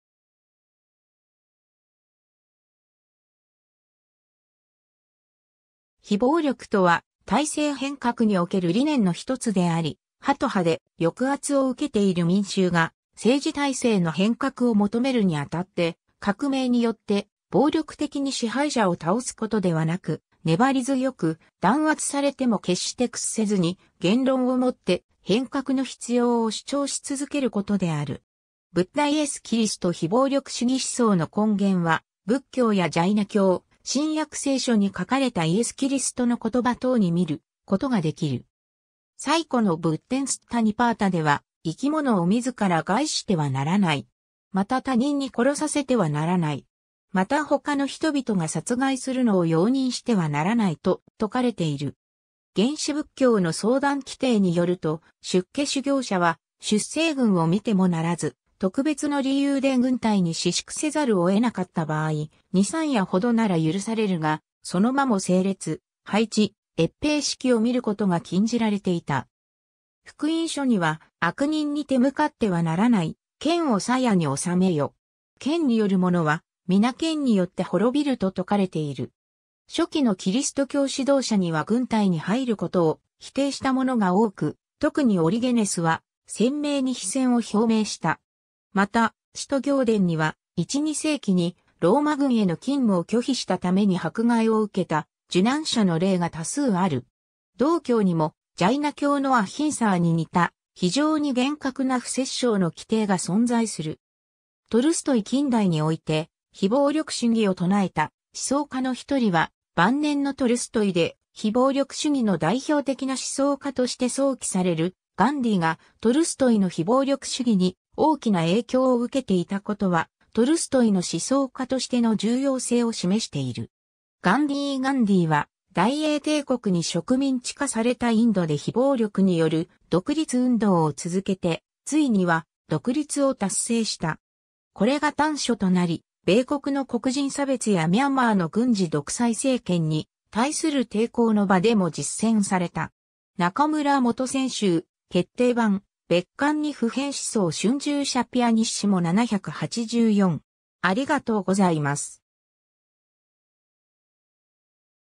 非暴力
非暴力（ひぼうりょく）とは、体制変革における理念のひとつであり、ハト派で、抑圧を受けている民衆が政治体制の変革を求めるにあたって、革命によって暴力的に支配者を倒すことではなく、粘り強く、弾圧されても決して屈せずに、言論を以って変革の必要を主張し続けることである。
 ブッダ イエス・キリスト 非暴力主義思想の根源は、仏教やジャイナ教、新約聖書に書かれたイエス・キリストの言葉等に見る事が出来る。
 最古の仏典スッタニパータでは「生きものをみずから害してはならない。また他人に殺させてはならない。また他の人々が殺害するのを容認してはならない」と説かれている。
原始仏教の僧団規定によると、出家修行者は出征軍を観てもならず、特別の理由で軍隊に止宿せざるを得なかった場合、二、三夜ほどなら許されるが、その間も整列・配置・閲兵式を観ることが禁じられていた。
 福音書には「悪人に手向かってはならない」「剣を鞘におさめよ。剣による者は、みな剣によって滅びる」と説かれている。
初期のキリスト教指導者には軍隊に入ることを否定した者が多く、特にオリゲネスは鮮明に非戦を表明した。
また使徒行伝には、一、二世紀にローマ軍への勤務を拒否したために迫害を受けた受難者の例が多数ある。
 道教にも、ジャイナ教のアヒンサーに似た、非常に厳格な不殺生の規定が存在する。
 トルストイ 近代において非暴力主義を唱えた思想家の一人は晩年のトルストイで、非暴力主義の代表的な思想家として想起されるガンディーがトルストイの非暴力主義に大きな影響を受けて居た事は、トルストイの思想家としての重要性を示している。
 ガンディー ガンディーは大英帝国に植民地化されたインドで非暴力（サティヤーグラハ）による独立運動を続けて、ついには独立を達成した。
これが端緒となり米国の黒人差別やミャンマーの軍事独裁政権に対する抵抗の場でも実践された。
 ^ 『中村元選集［決定版]　別巻２　普遍思想』 春秋社　pp784